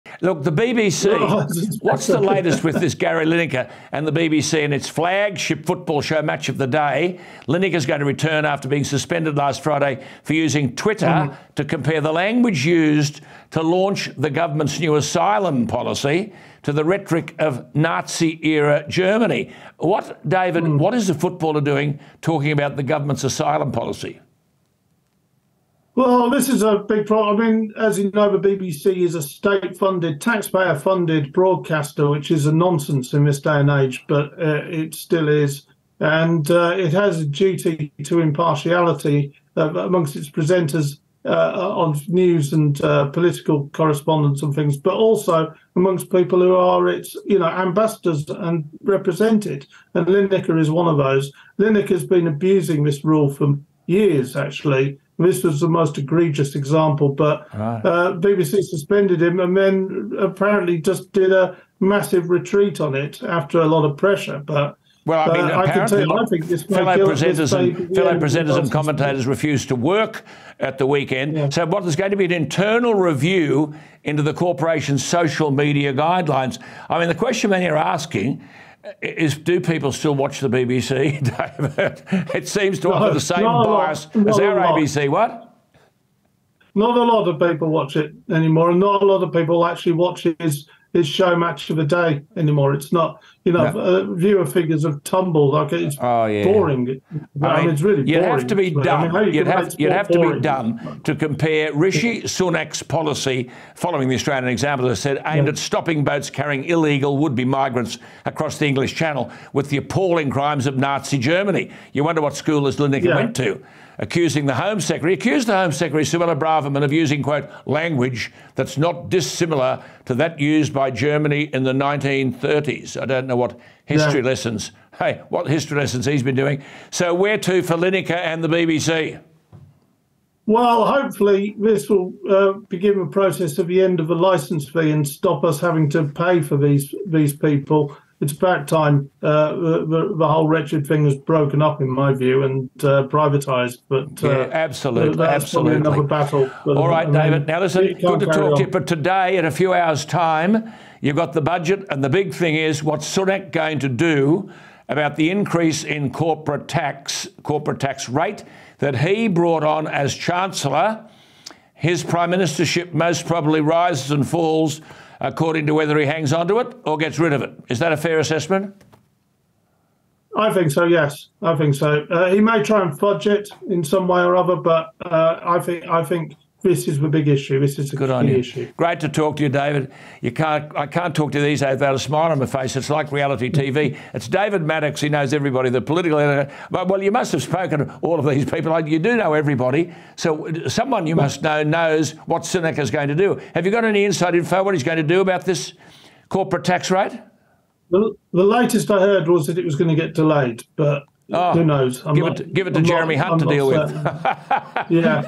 Gary Lineker should keep his nose well out of politics: David Maddox | Alan Jones
‘Lineker’s been abusing this rule for years actually and this is the most egregious example.’

Political editor of the Express Online David Maddox joins Alan Jones.